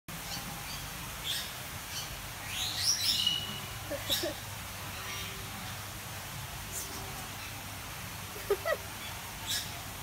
Зоопарк Шошто в Ньиредьхазе - обезьяна
Увидев нас, а скорее наши фотоаппараты, обезьяна подбежала к стеклу (стекло-это барьер между жителем и посетителем зоопарка), села в фотогеничную позу и не сдвинулась с места пока мы не отошли. А как только она поняла, что больше не в центре внимания, сразу стала зевать и чесаться, понимая, что мы сразу же опять к ней вернемся.